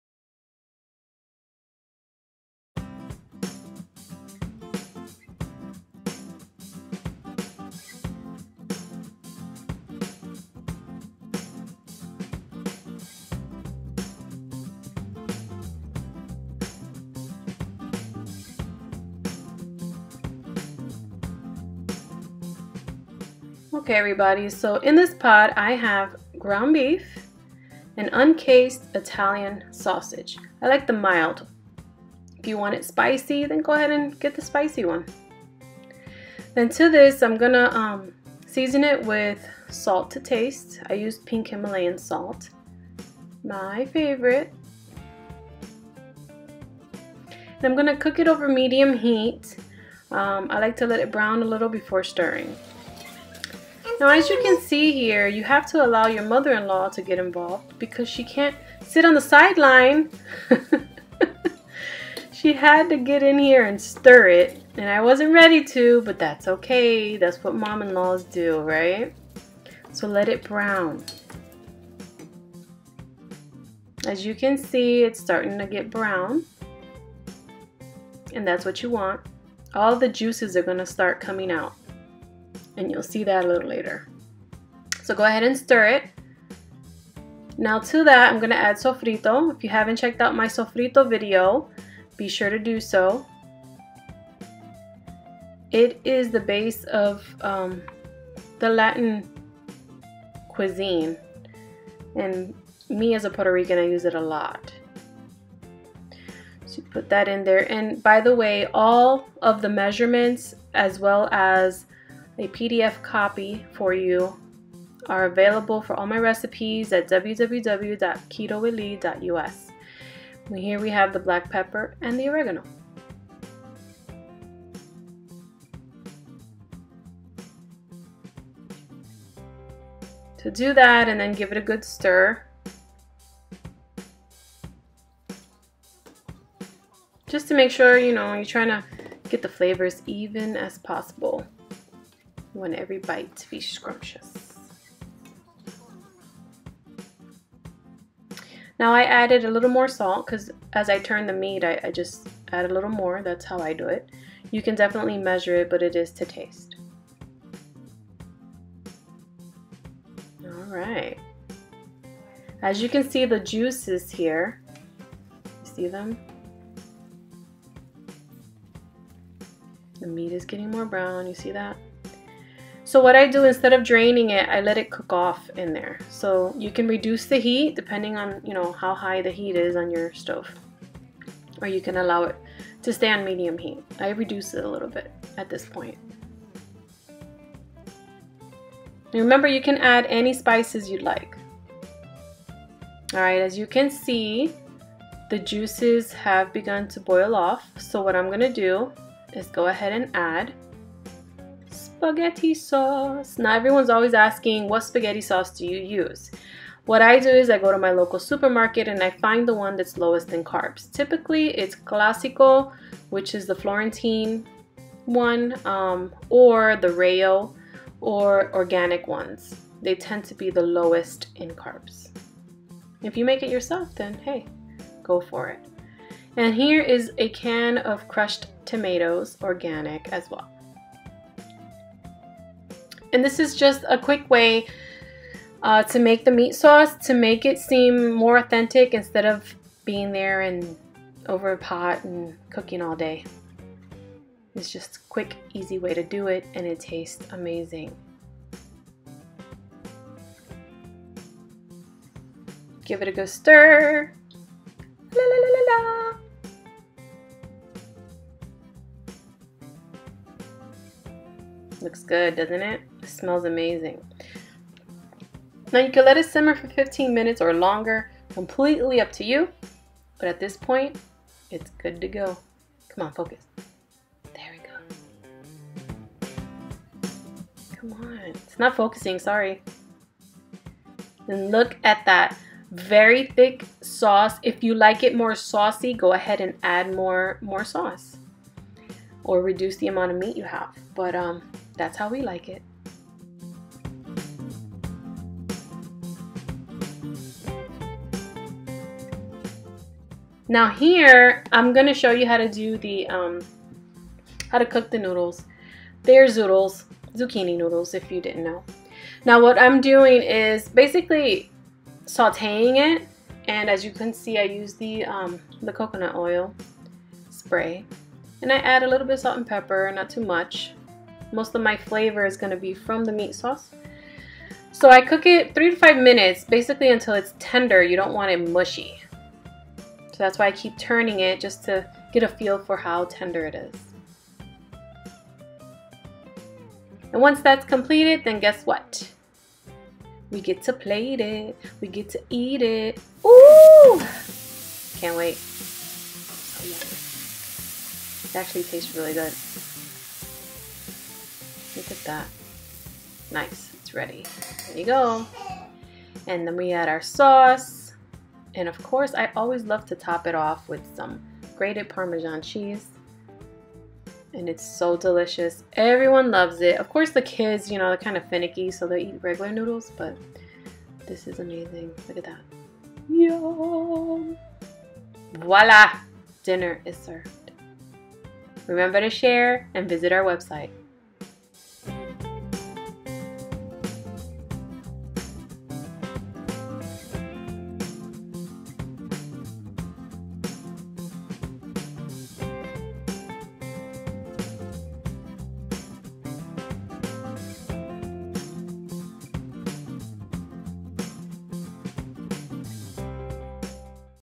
Keto Friendly Recipe - The Greatest Low Carb Meat Sauce Ever
Don't know what to make dinner? Try this easy recipe that the entire family will love.

Written recipe:
Ingredients:
-1 pound– Ground beef / Ground turkey
-1 pound– Un-Cased Mild/Hot Italian Sausage
-1 Tablespoon– Sofrito
-1 teaspoon– Dominica Oregano (or whatever oregano you have available)
-1 teaspoon– black pepper Salt to your taste
-1 (24 ounce) container–Your Favorite Low-Carb Spaghetti Sauce
-1 (28 ounce) can– Organic Crushed Tomatoes
-Coconut, Avocado or Olive oil — non-stick spray
-Zucchini’s — 1 per person you are feeding

Instructions:
1. Pre-heat your oven to 375*F
2. Make sure your pork tenderloin is brought to room temperature. Transfer your tenderloins to a baking dish.
3. In a small bowl combine the following: lemon juice, minced garlic, salt, black pepper, olive oil and oregano. Stir until well combined.
 4. Pour mixture over the pork tenderloin. Try to cover all sides.
 5. Allow to sit and marinate for 20-30 minutes. Overnight is soooo much better!
 6. Drain the excess juices. The tenderloin will release it’s own juices.
 7. Bake at 375*F for 35-45 minutes. Baste the meat with it’s own juices.
 8. Check the temperature to ensure it is cooked thoroughly. Perfect temperature for pork is 170*F.
 9. In my case, I had to return it to the oven and bake it for another 20 minutes. Every oven is different and varies with the temperature.
 10. Broil on High for 5 minutes or until golden brown.

?? How To Lose 41lbs in 4 Weeks?